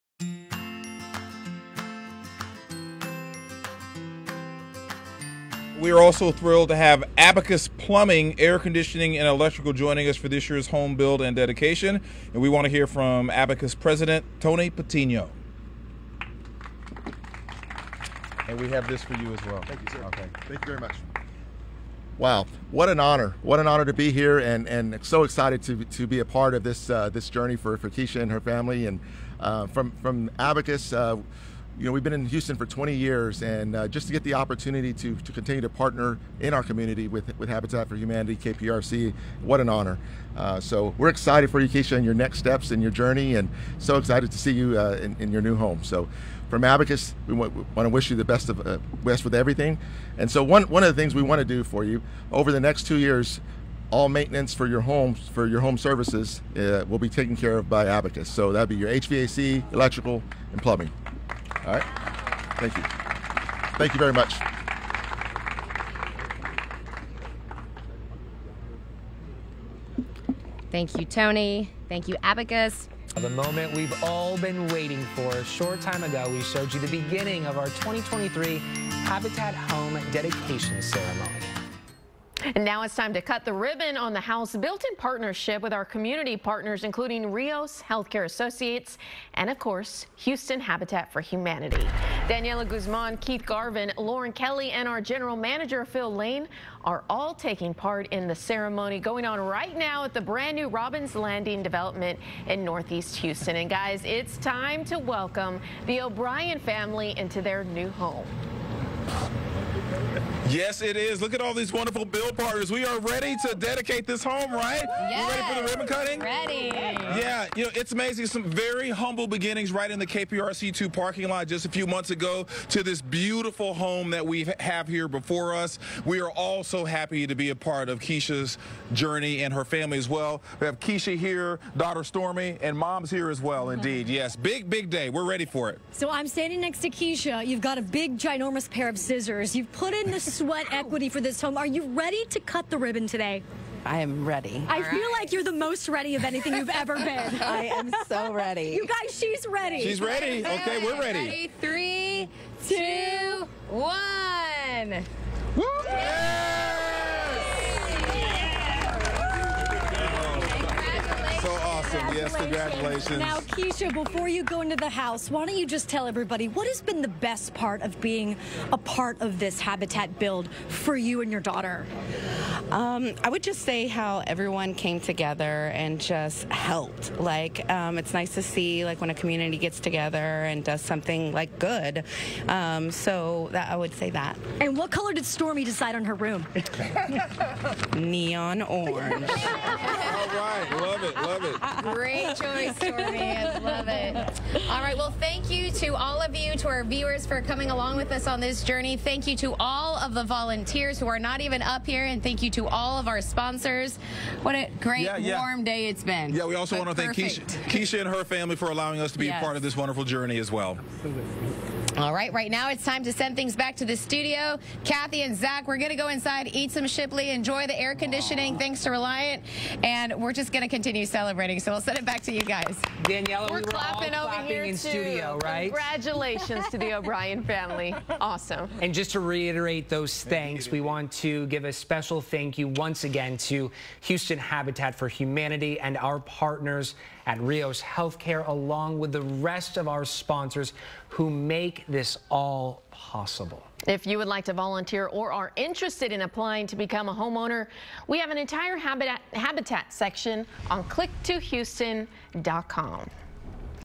Abacus Helps With Habitat for Humanity Ribbon Cutting In Northeast Houston
Abacus Helps With Habitat for Humanity Ribbon Cutting In Northeast Houston. Tony Patino addresses the crowd for the dedication and ribbon cutting  of the New Habitat Home.

Speaker 1:
We are also thrilled to have Abacus Plumbing, Air Conditioning and Electrical joining us for this year's home build and dedication. And we want to hear from Abacus President Tony Patiño. And we have this for you as well.

Tony Patiño:
Thank you very much. Wow. What an honor to be here and so excited to be a part of this journey for Keisha and her family. And from Abacus, we've been in Houston for 20 years and just to get the opportunity to continue to partner in our community with Habitat for Humanity, KPRC. What an honor. So we're excited for you Keisha and your next steps in your journey, and so excited to see you in your new home. So from Abacus, we want to wish you the best with everything. And so, one of the things we want to do for you over the next two years, all maintenance for your home services will be taken care of by Abacus. So that'd be your HVAC, electrical and plumbing. All right, thank you. Thank you very much.

Keshia O'Brian:
Thank you. Thank you. [inaudible 00:01:11]

Daniella Guzman:
Thank you, Tony. Thank you, Abacus.

Zach:
And the moment that we've all been waiting for. Short time ago, we showed you the beginning of our 2023 Habitat Home dedication ceremony.

Cathy:
And now it's time to cut the ribbon on the house. Daniella Guzman, Keith Garvin, Lauren Kelly, and our General Manager Phil Lane are all taking part in the ceremony going on right now at the brand-new Robins Landing Development in Northeast Houston. And guys, it's time to welcome the O'Brian family into their new home.

Speaker 1:
Yes, it is. Look at all these wonderful build partners. We are ready to dedicate this home, right?

Daniella Guzman:
Yes.

Speaker 1:
We ready for the ribbon cutting?

Daniella Guzman:
Ready.

Keshia O'Brian:
Yes.

Speaker 1:
Yeah. It's amazing. Some very humble beginnings right in the KPRC 2 parking lot, just a few months ago to this beautiful home that we have here before us. We are also happy to be a part of Keisha's journey and her family as well. We have Keisha here, daughter Stormy and mom's here as well, indeed. Yes. Big, big day. We're ready for it.

Speaker 7:
So I'm standing next to Keisha. You've got a big ginormous pair of scissors. You've put in the sweat equity for this home. Are you ready to cut the ribbon today?

Keshia O'Brian:
I am ready.

Speaker 7:
I feel like you're the most ready of anything you've ever been.

Keshia O'Brian:
I am so ready.

Speaker 7:
You guys, she's ready.

Speaker 1:
She's ready. Okay, we're ready.

Daniella Guzman:
[inaudible 00:02:31] Ready? Three, two, one.

Speaker 7:
Two.

Speaker 1:
Yes.

Daniella Guzman:
Congratulations.

Speaker 1:
So awesome. Yes. Congratulations.

Speaker 7:
Congratulations. Now, Keisha, before you go into the house, why don't you just tell everybody what has been the best part of being a part of this habitat build for you and your daughter?

Keshia O'Brian:
I would just say how everyone came together and just helped. It's nice to see when a community gets together and does something good. So I would say that.

Speaker 7:
And what color did Stormy decide on her room?

Keshia O'Brian:
Neon orange.

Speaker 1:
All right. Love it. Love it.

Daniella Guzman:
Great choice, Stormy. I love it. All right. Well, thank you to all of you. To our viewers for coming along with us on this journey. Thank you to all of the volunteers who are not even up here. What a great warm day it's been.

Speaker 1:
Yeah. We also want to thank Keisha and her family for allowing us to be a part of this wonderful journey as well.

Speaker 8:
Absolutely.

Daniella Guzman:
All right. Right now, it's time to send things back to the studio. Cathy and Zach, we're going to go inside, eat some Shipley, enjoy the air conditioning, thanks to Reliant. And we're just going to continue celebrating. So we'll send it back to you guys.

Zach:
Daniella, we were all clapping in studio.

Cathy:
We're clapping over here in studio.

Zach:
Right?

Cathy:
Congratulations to the O'Brian family. Awesome.

Zach:
And just to reiterate those thanks, we want to give a special thank you once again to Houston Habitat for Humanity and our partners at Rios Healthcare along with the rest of our sponsors who make this all possible.

Cathy:
If you would like to volunteer or are interested in applying to become a homeowner, we have an entire habitat section on Click2Houston.com.